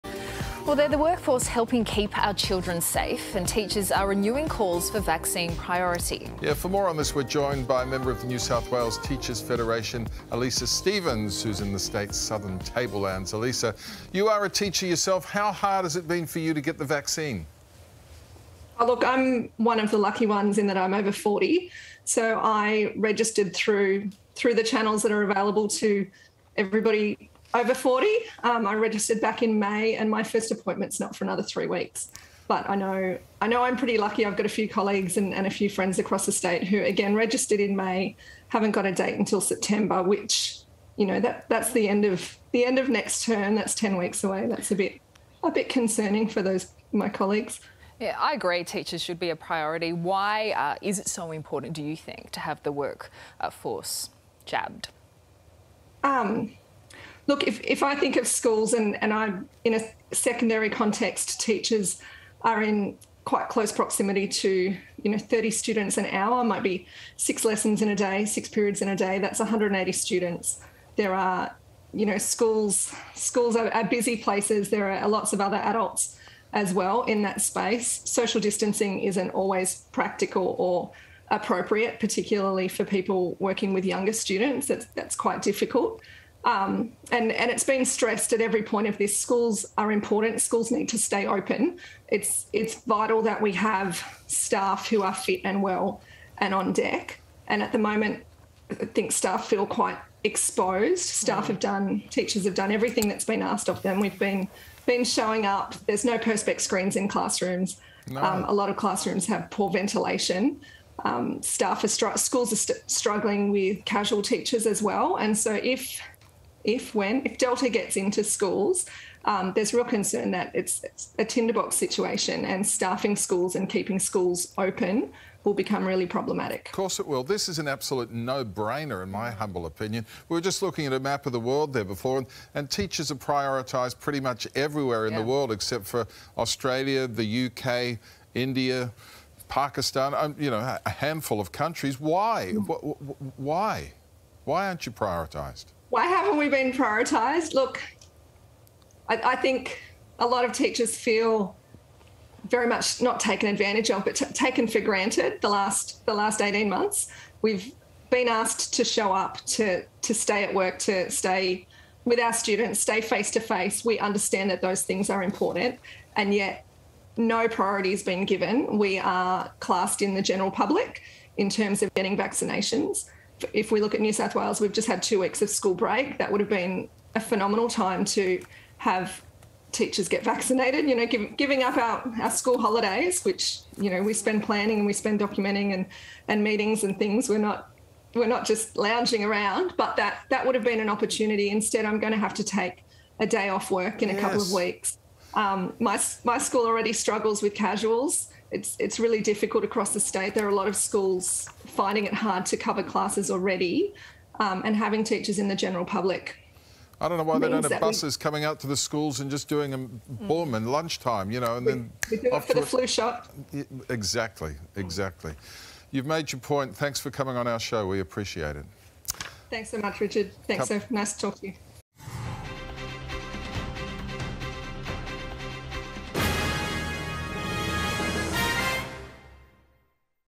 Teachers call for COVID-19 vaccine priority | Coronavirus | 9 News Australia
School staff are pushing for coronavirus vaccine priority, after fears it could be ‘a tinderbox situation’ if the virus enters the school system.